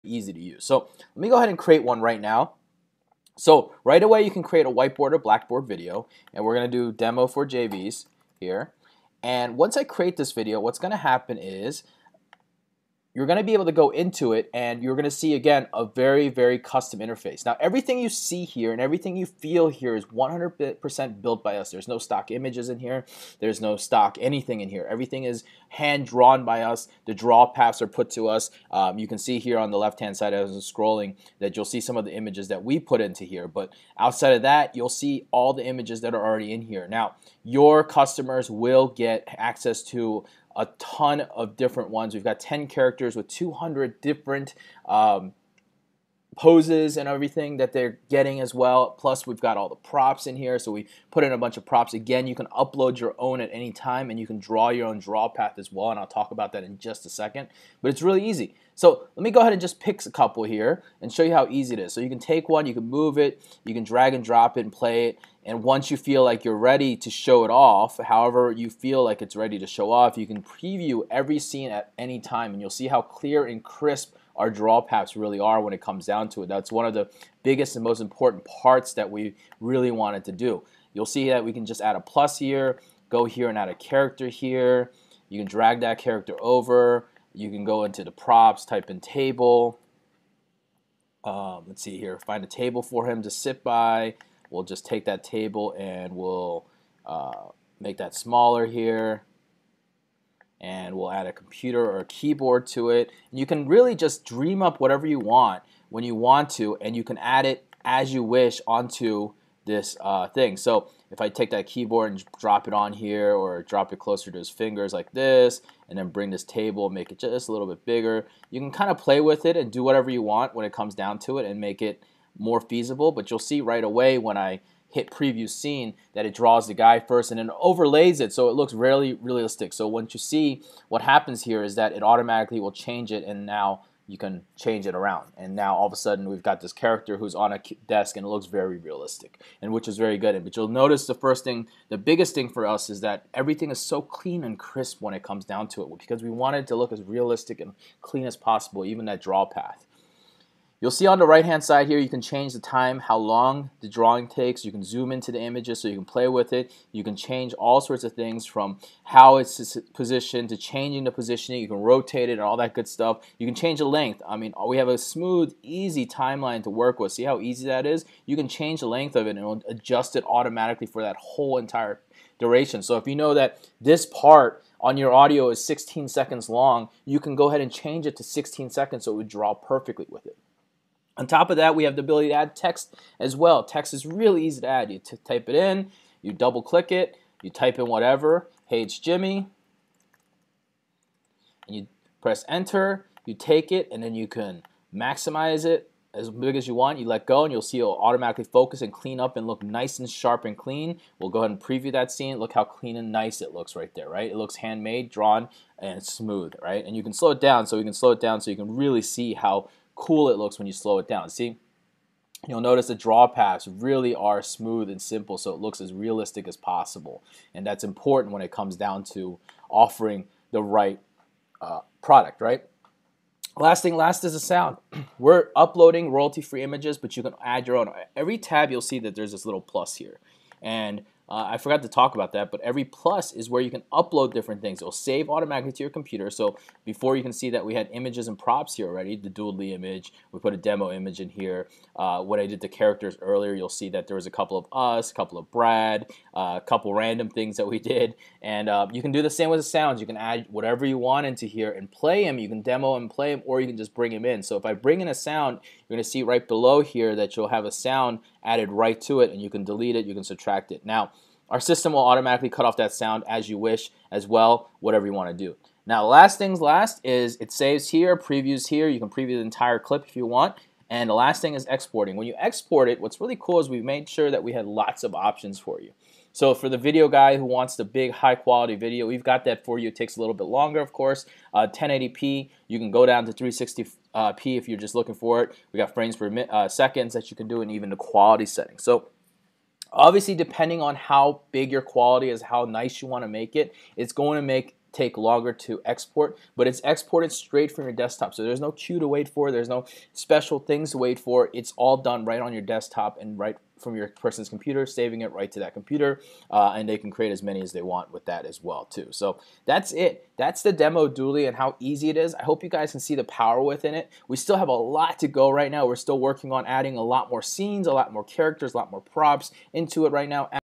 Doodly Review - Hottest Video Creator App
What Is Doodly?

Doodly is a powerful point and click desktop software that allows

ANYONE to create doodle style videos with an easy to use drag and

drop interface.

Included is over 200 custom “poses”, 20 scenes, 200 props AND the

ability to upload your own images.

Fully tested and developed from the ground up, Doodly allows users to

create unlimited videos right from their desktop for their marketing

needs.

Doodly is the newest & hottest video creator app to hit the market:

– An easy to user interface that is newbie & tech friendly
– Drag & drop/Push Button ease!
– Includes EVERYTHING you need to get started & create your 1st video

today
– Create White board & Blackboard videos!
– Includes a strong variety of done for you images
– Lets custom image to upload with custom draw paths
– Lets you create UNLIMITED number of videos
– Available for both PC & Mac (desktop software)

Doodly
Doodly review
Doodly software